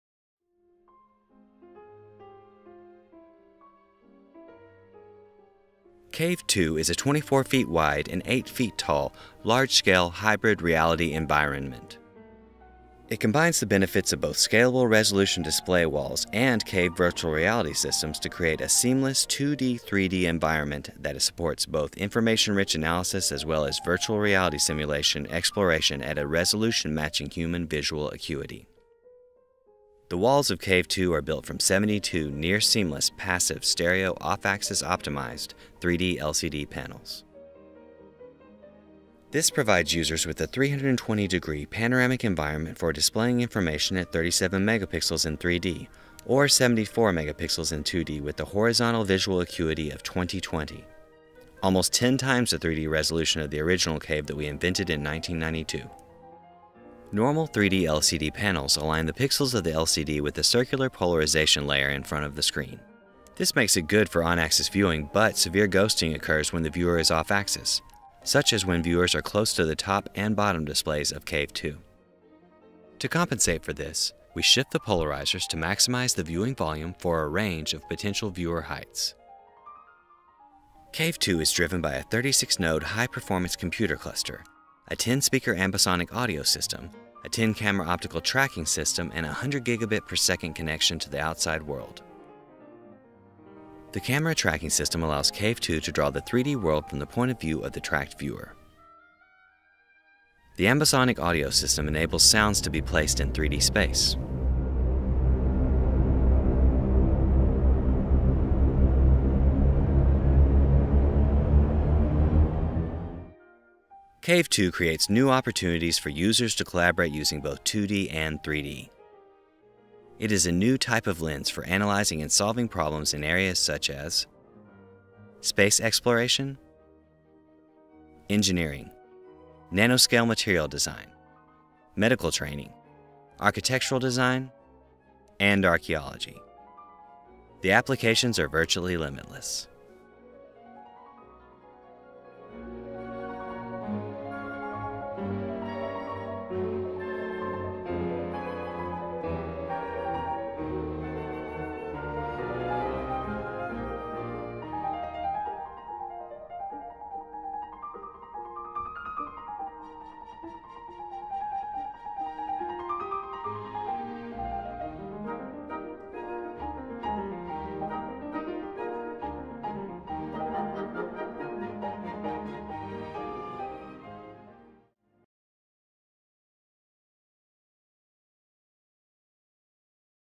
CAVE2™ Hybrid Reality Environment Documentary
CAVE2 - Hybrid Reality Environment
Electronic Visualization Laboratory, University of Illinois at Chicago

CAVE2 is a 24 feet wide and 8 feet tall large scale hybrid reality environment.

It combines the benefits of both scalable-resolution display walls and CAVE virtual-reality systems to create a seamless 2D/3D environment, that supports both information-rich analysis as well as virtual-reality simulation exploration, at a resolution matching human visual acuity.

The walls of CAVE2 are built from 72 near-seamless passive stereo off-axis-optimized 3D LCD panels.

This provides users with a 320-degree panoramic environment for displaying information at 37 Megapixels in 3D or 74 Megapixels in 2D with a horizontal visual acuity of 20/20 -- almost 10 times the 3D resolution of the original CAVE that we invented in 1992.

Normal 3D LCD panels align the pixels of the LCD with the circular polarization layer in front of the screen.

This makes it good for on-axis viewing but severe ghosting occurs when the viewer is off-axis, such as when viewers are close to the top and bottom displays of CAVE2.

To compensate for this we shift the polarizers to maximize the viewing volume for a range of potential viewer heights.

CAVE2 is driven by a 36-node high-performance computer cluster, a 20-speaker ambisonic audio system, a 10-camera optical tracking system and a 100-Gigabit/second connection to the outside world.

The camera tracking system allows CAVE2 to draw the 3D world from the point of view of the tracked viewer.

The ambisonic audio system enables sounds to be placed in the 3D space.

CAVE2 creates new opportunities for users to collaborate using both 2D and 3D. It is a new type of lens for analysing and solving problems in areas such as space exploration, engineering, nanoscale material design, medical training, architectural design and archeology. The applications are virtually limitless.